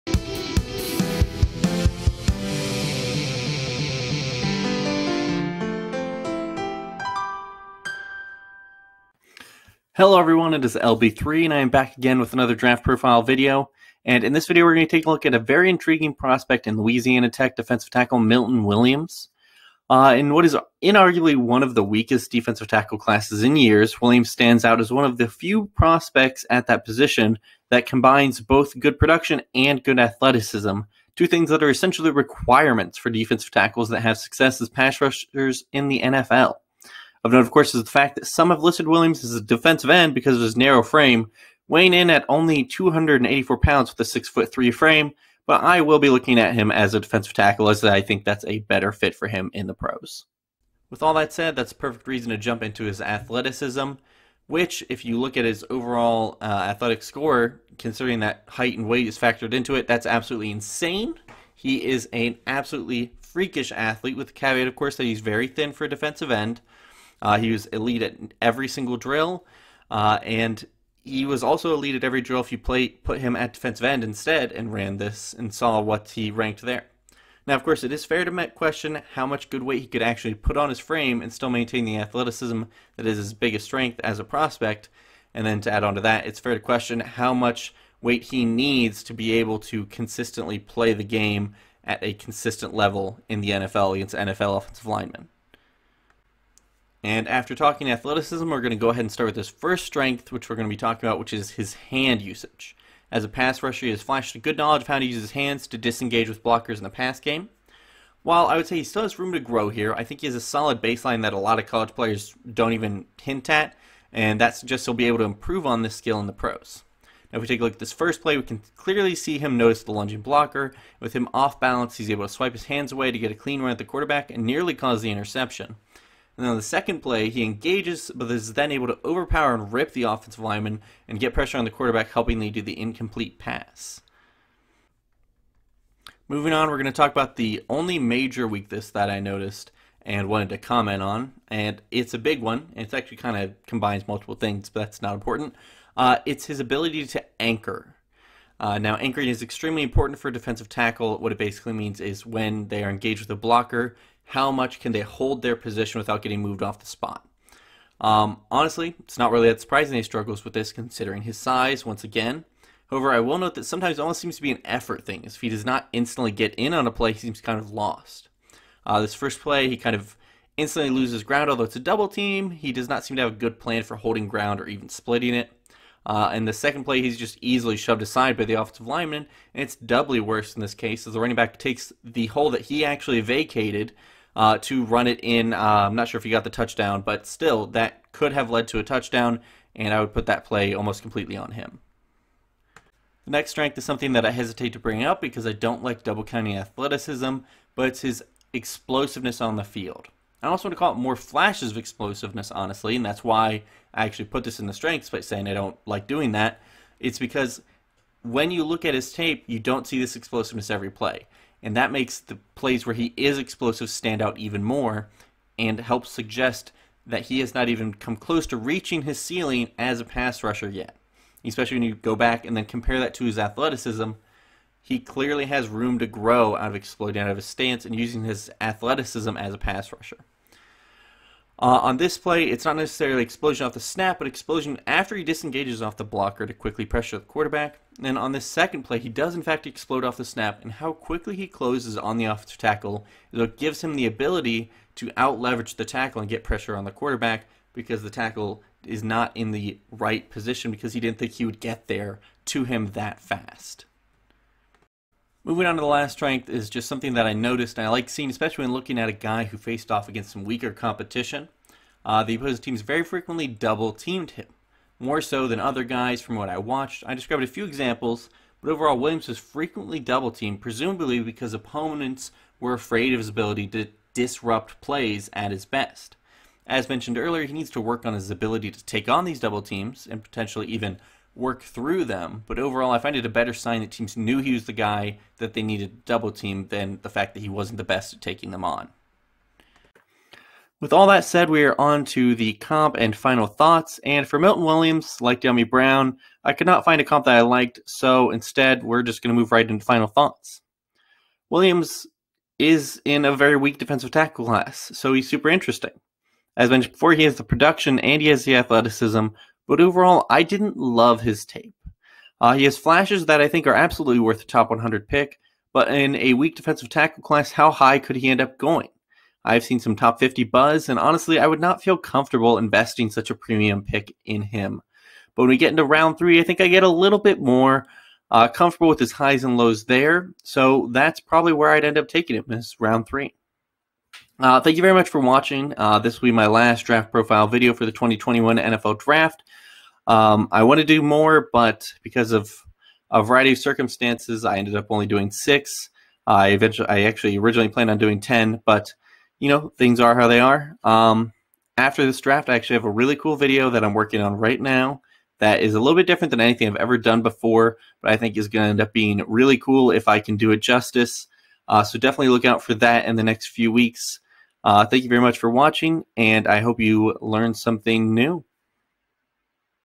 Milton Williams 2021 NFL Draft Profile Video
In this Draft Profile we take a look at and break down Lousiana Tech Defensive Tackle, a prospect with an intriguing ceiling at a weak position for the 2021 NFL Draft.